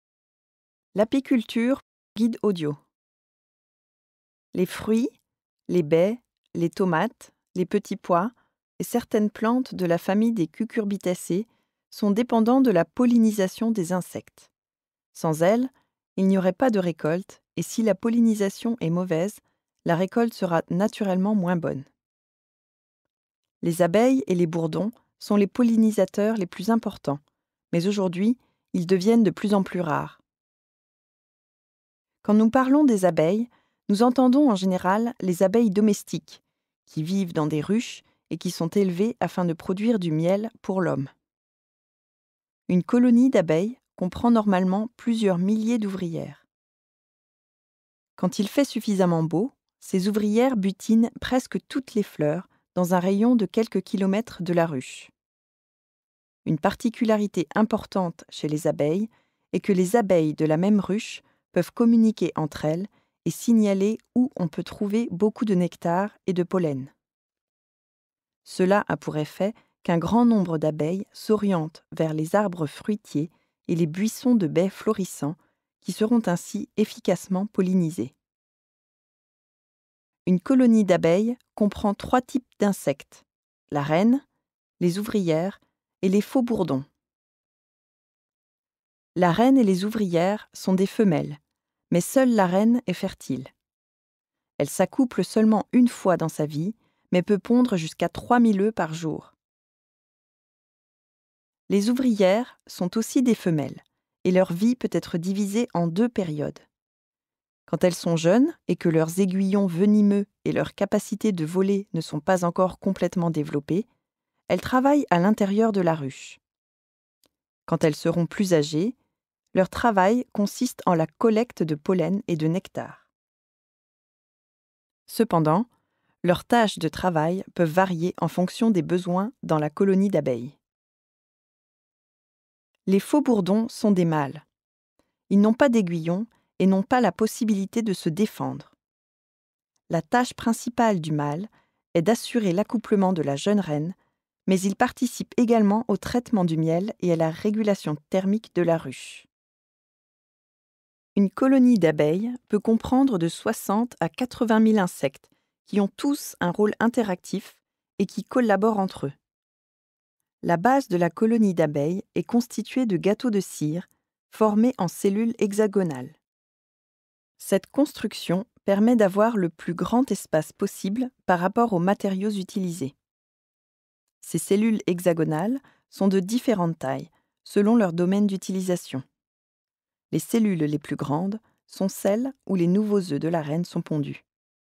15 L’apiculteur de Badelundaåsen détaillé
Ce film audio fait partie des guides audio appartenant au sentier culturel et de découverte de Badelundaås à Anundshög, à l’est de Västerås.